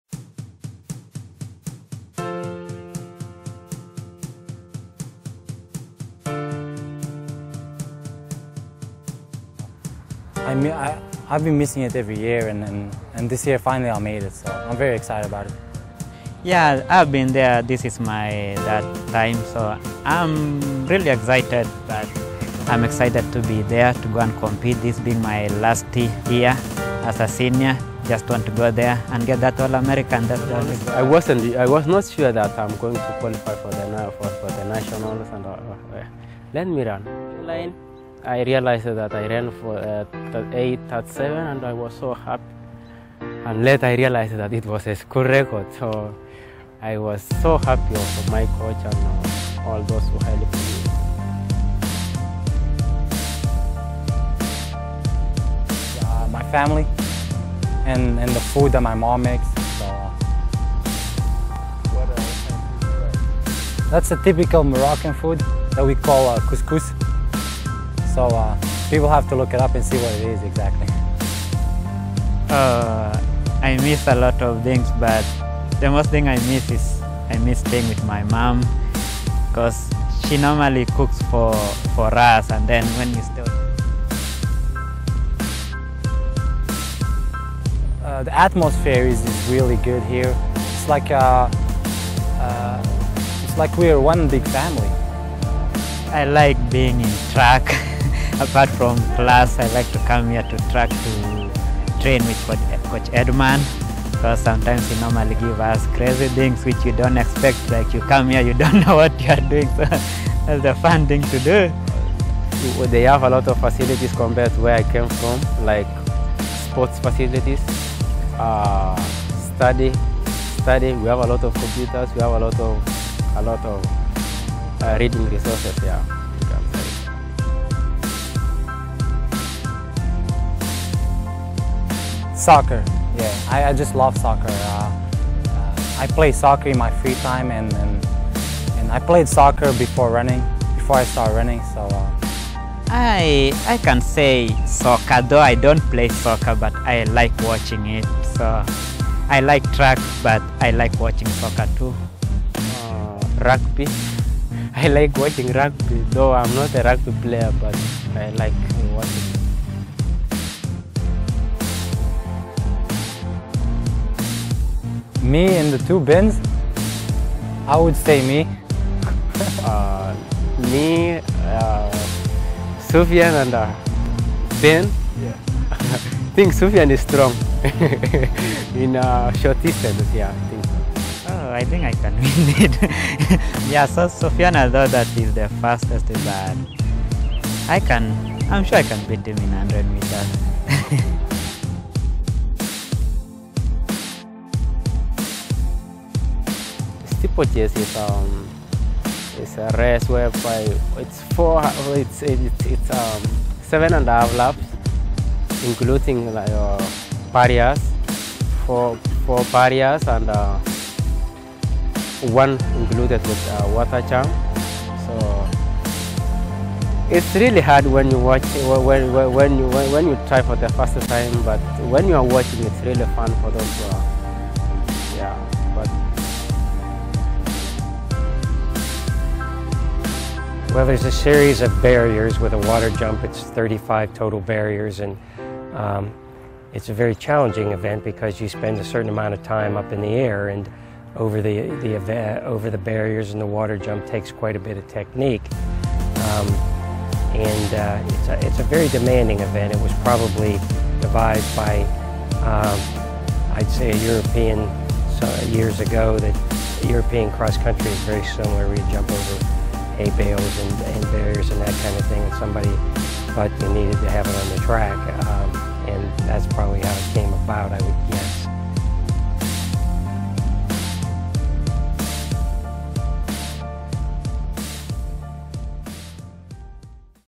2012 NCAA Track and Field Preview
Get to know EKU's three NCAA Track and Field qualifiers: Soufiane Bouchikhi, Ben Cheruiyot and Ben Toroitich.